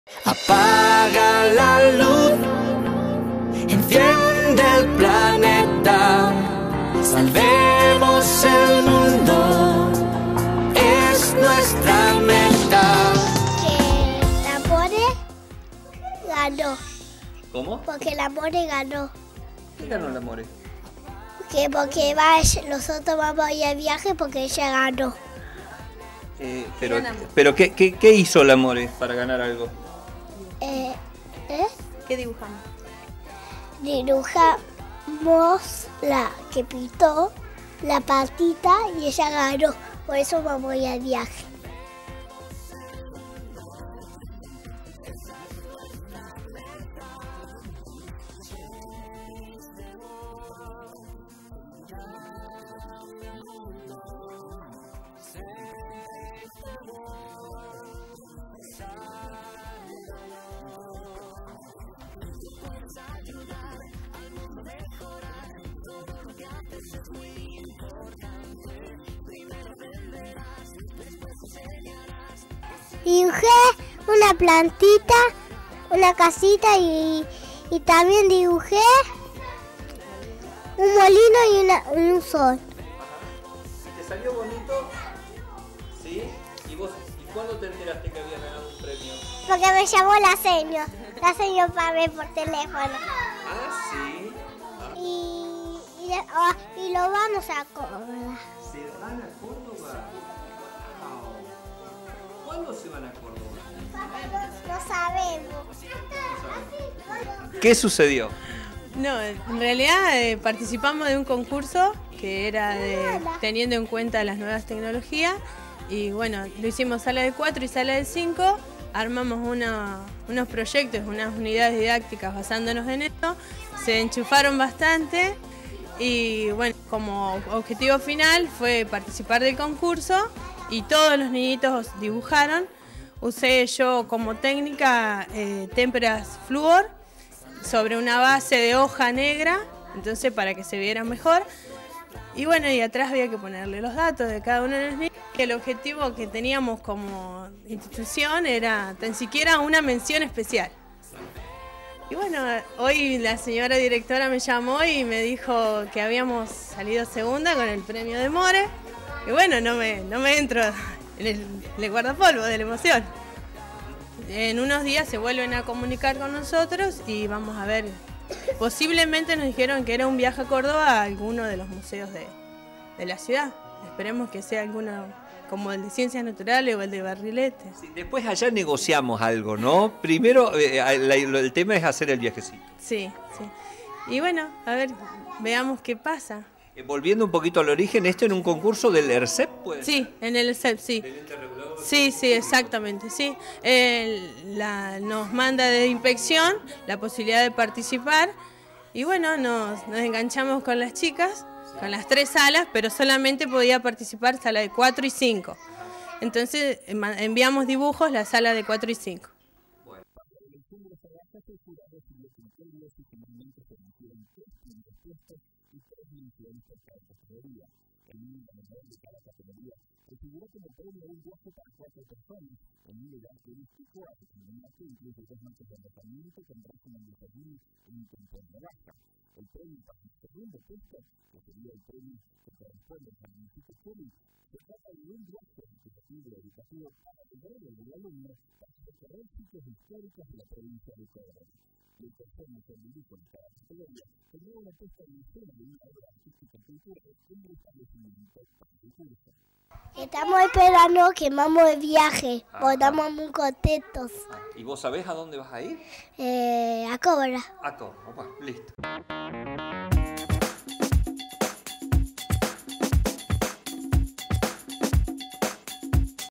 PREMIAN TRABAJO DE ALUMNA DEL JARDIN JUANA J. CHELI
La pequeña Morena Ponce, alumna de Nivel Inicial, participó del concurso "Cuidemos La Luz" implementado por el ERSEP con el objetivo de concientizar sobre el consumo racional de la energía eléctrica.
Tras el cierre del concurso y la observación de los trabajos presentados, Morena obtuvo el segundo premio en la Categoría Mini y aguarda junto a sus compañeritos disfrutar del mismo que consiste en  un viaje recreativo/educativo para el grado del alumno, para recorrer sitios históricos de la Provincia de Córdoba.